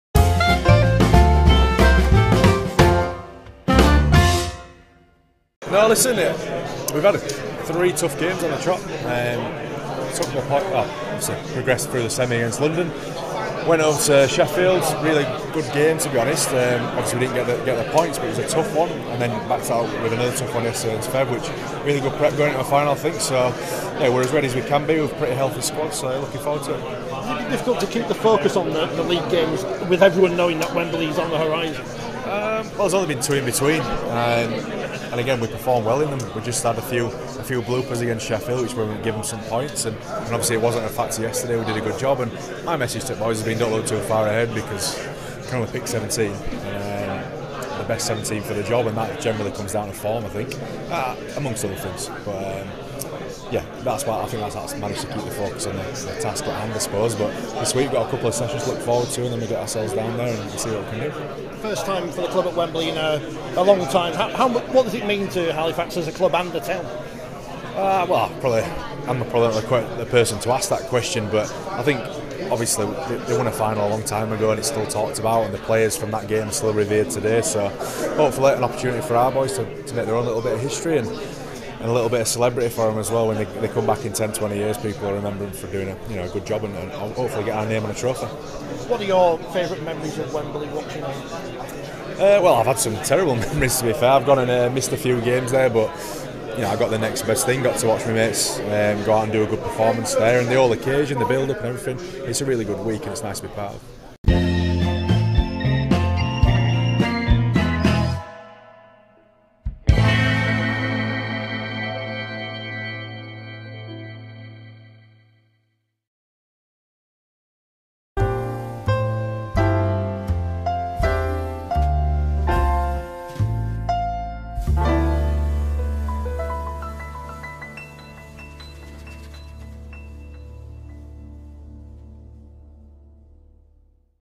Simon Grix on Halifax's 1895 Cup Final with Batley - Forty20 TV
When Halifax were last at Wembley, the Blue Sox hadn't been thought of, let alone Panthers, as they became the first team beaten by Wigan on their famous run of wins at the stadium.

They return in 2023 to face Batley Bulldogs in the final of the 1895 Cup, and coach Simon Grix tells us about the challenge that awaits them.